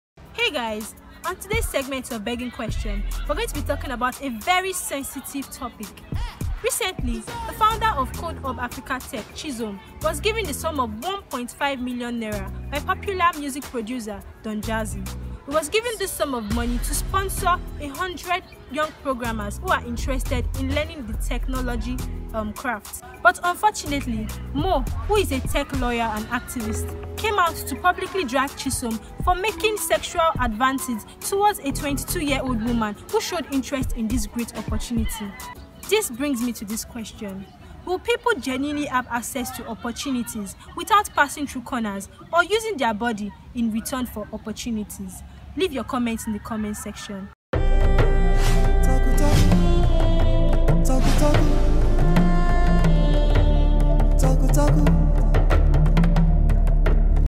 When will women have equal access to opportunities without sexual requests in return?
Chizom, the founder of Code Hub Africa Tech, was given the sum of 1.5 million Naira bypoular Nigerian record producer and musician, Don-jazzy, to sponsor one hundred young programmers, he found himself being publicly called out on social media by revolutionary tech lawyer and activist, Moe, for proposing sex to a 22 year old young woman who ventured into his DM to inquire about acquiring a scholarship to be a part of his program. This brings the begging question for the day. Will people genuinely be able to access opportunities without having to pass through corners or giving their bodies in return? Leave your comments in the comment section